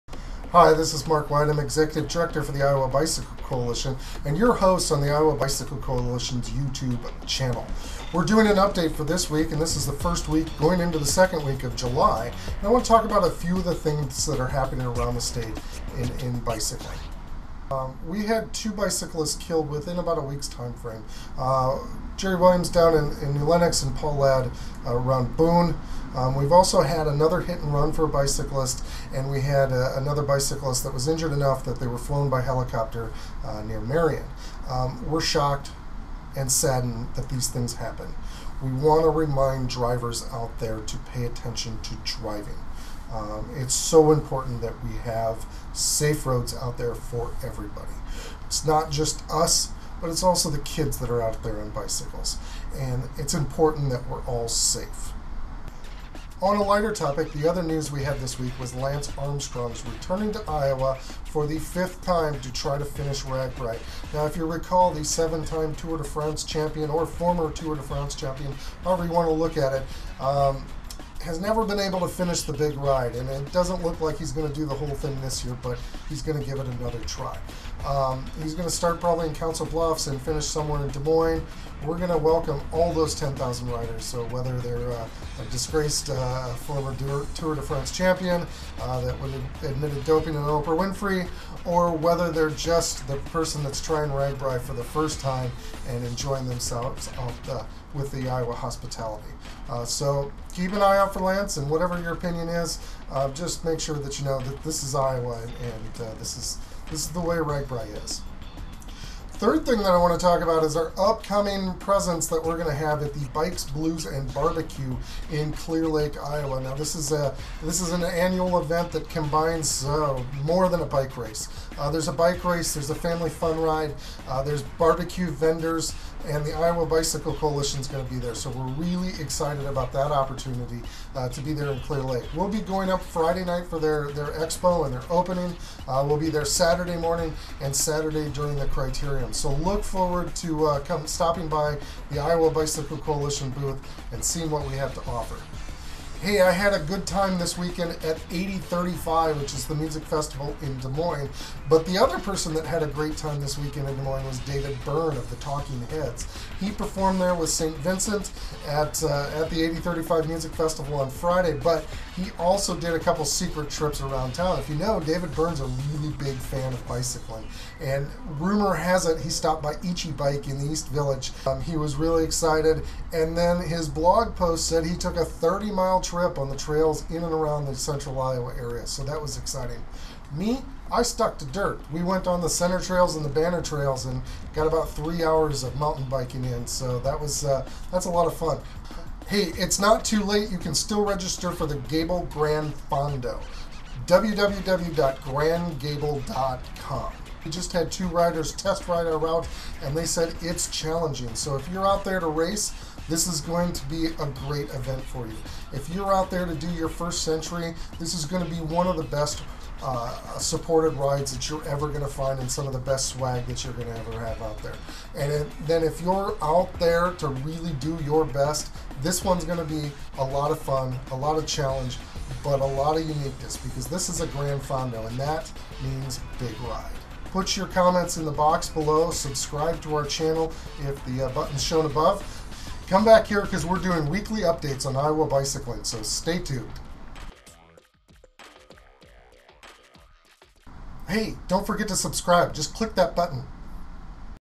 Iowa Bike Advocacy Update July 2013
Two bike fatalities.  Please be careful driving.
Lance to ride RAGBRAI
Clear Lake Bikes Blues and BBQ
David Byrne rides in Iowa